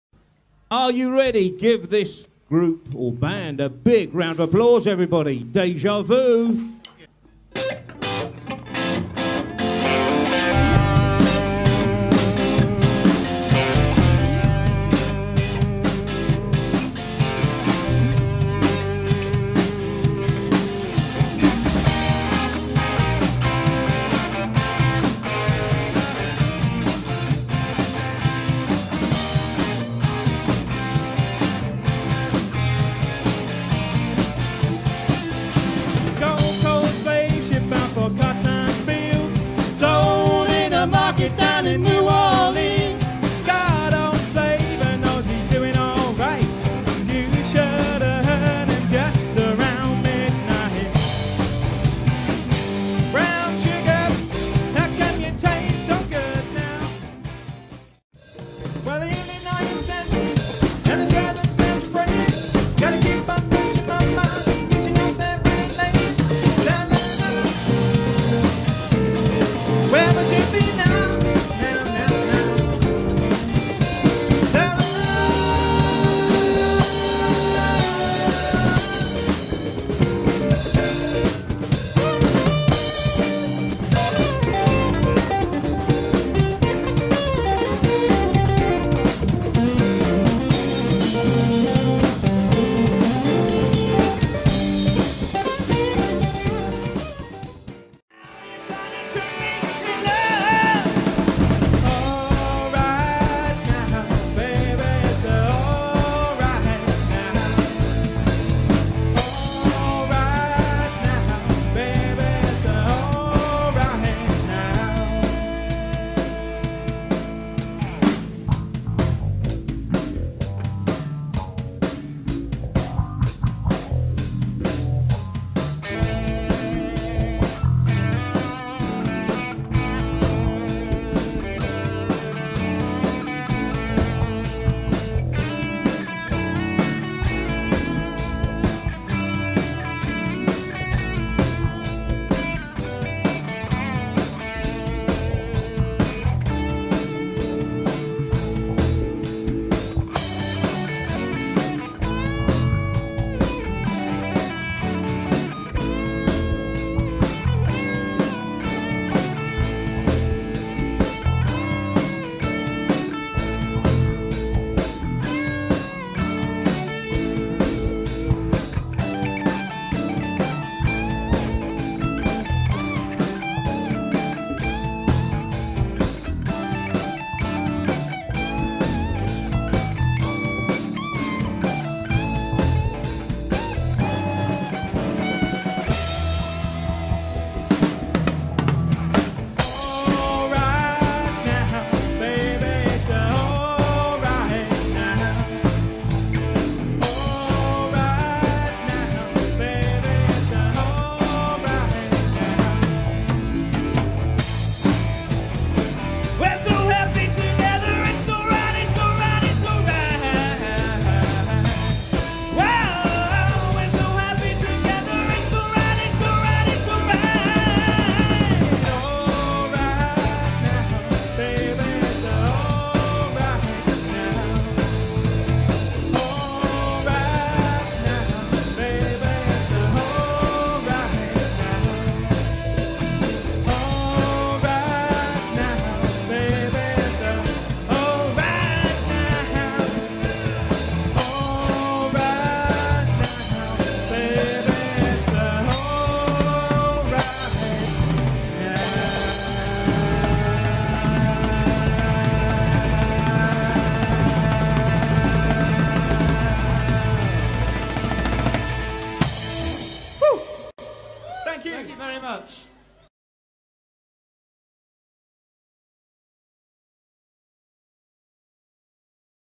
Deja Vu - Live in the Park!
A collection of videos, audio and photos of Deja Vu, performing LIVE at the Huntingdon Carnival 'Live in the Park' event, recorded live on 13th August 2017.

Many thanks to HCR104fm for the audio, and family and friends for the videos and pictures.